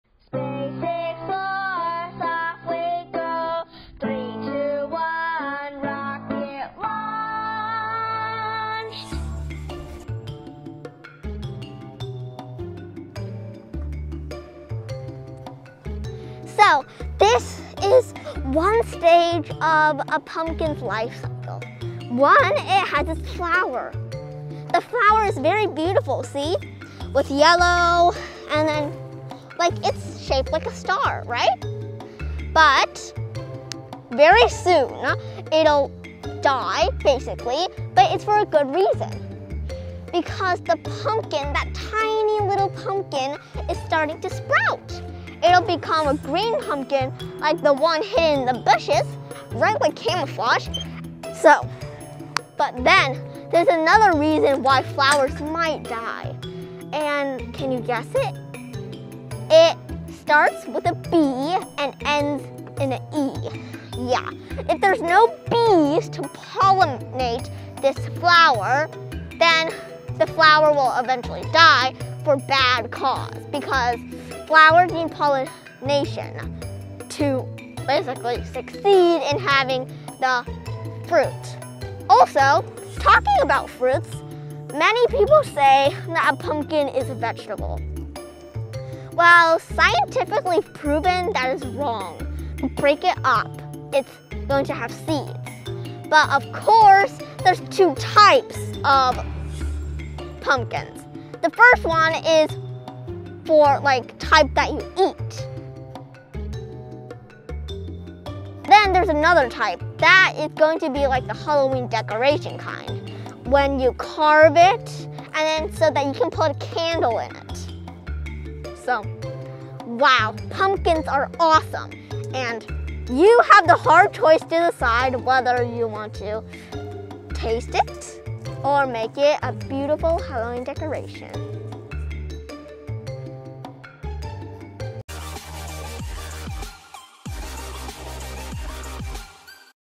What cause Pumpkin Flowers to wilt? Taste or Decorate a Pumpkin? | Pumpkin Facts For Kids
Hi I am a Big Big fan of Our Nature as well as Our Universe! Today let's explore pumpkin flowers and pumpkin types! What cause pumpkin flowers to wilt? Choose a pumpkin for taste, or choose a pumpkin for decoration? Space Explorers off we go..3,2,1...rocket launch!